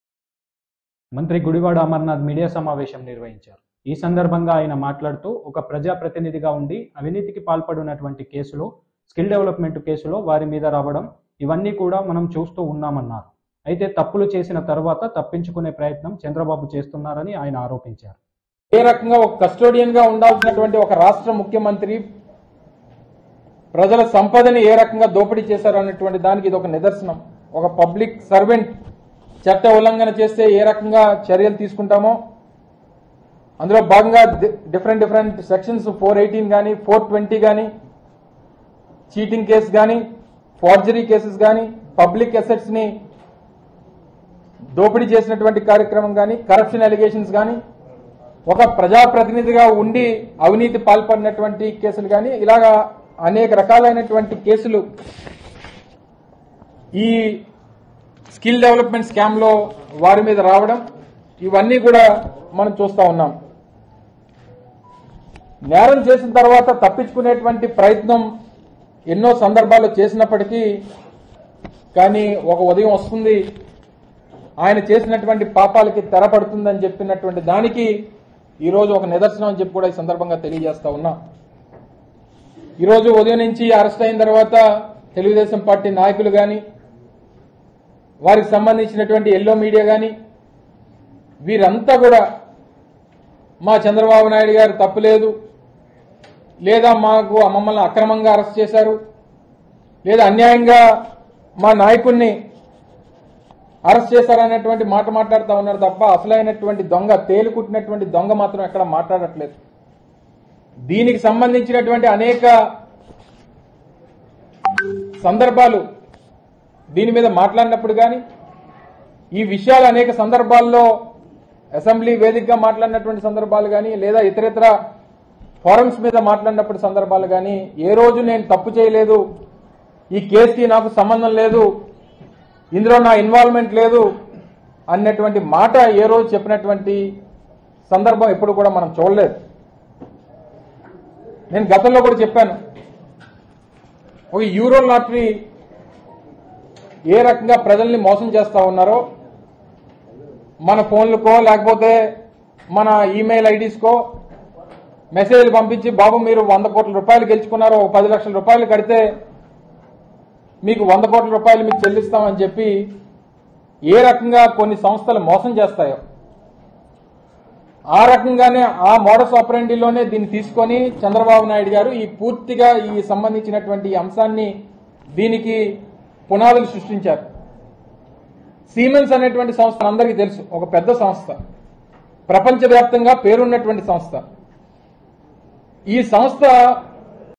Minister Gudivada Amarnath about Chandrababu Arrest || TODAY NEWS
Gudivada Amarnath Fire on Pawan Kalyan,Purandeswari | Chandrababu Arrest  || TODAY NEWS ||